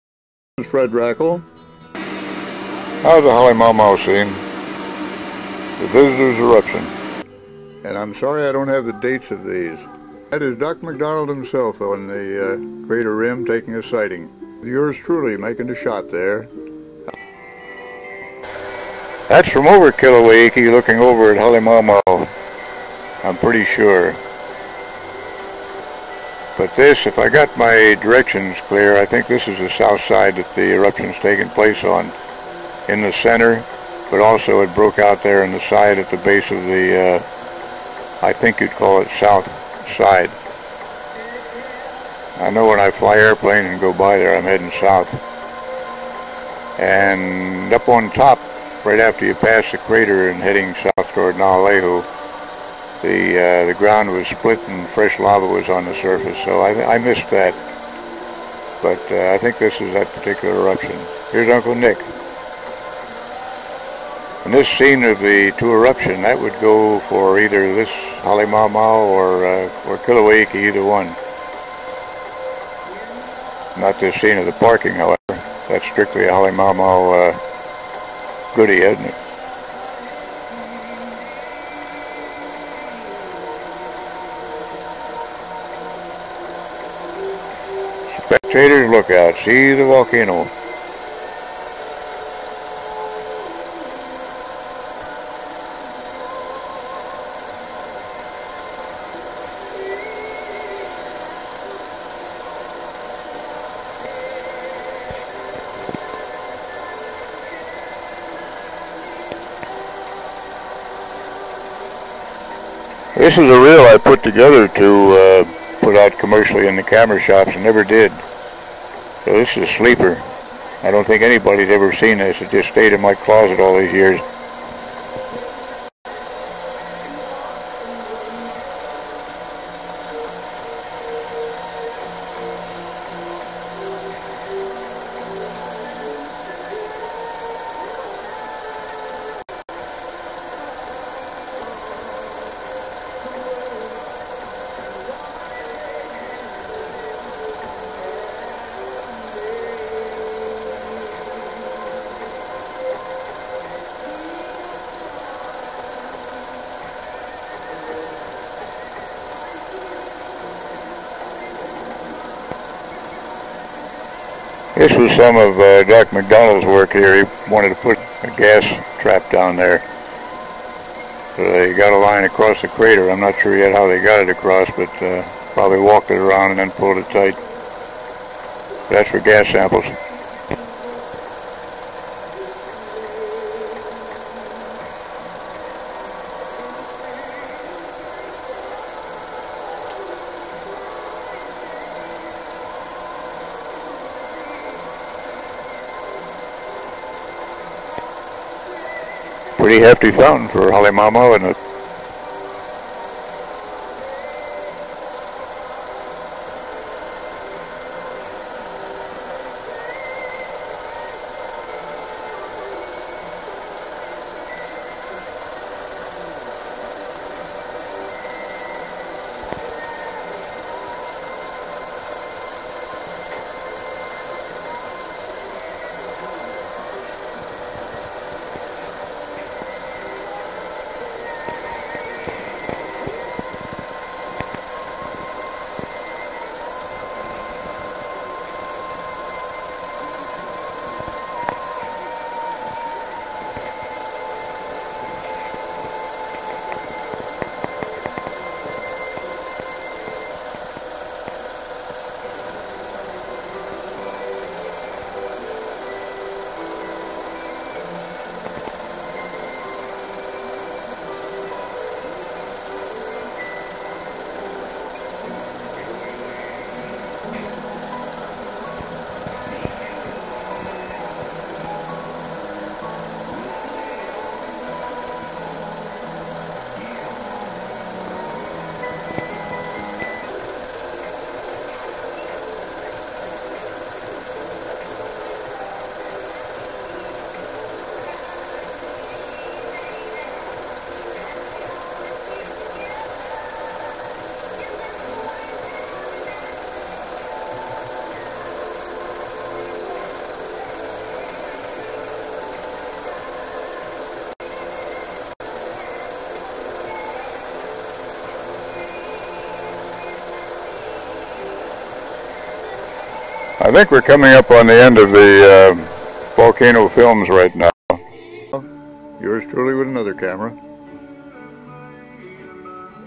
CSAV Hawaii: Halemaumau eruption 1961, Rackle
Historic 16mm footage of Halemaumau erupting in 1961. Fred Rackle filmed this eruption, shortly after he documented the 1959 & 1960 eruptions of Kilauea Iki and Kapoho. In 1994, Rackle set up a 16mm projector in his living room in Makaha Towers, and taped the decades-old film footage with a VHS camera while he narrated. Then he donated the VHS tapes to CSAV. At that time, everyone was most interested in the Kilauea Iki and Kapoho tapes. But now Halemaumau is once again in the spotlight, and perhaps this footage will be useful to volcanologists. The footage of fountains in Halemaumau is almost certainly the July 10, 1961 eruption, the longest (7 days) of the 3 summit eruptions in 1961. Fred Rackle was a brilliant photographer, not only because he had an eye for beauty, but because he worked alongside scientist Gordon Macdonald, who was Chief Scientist of HVO at that time; Macdonald helped Rackle see what was of scientific interest. Also, Rackle used a tripod.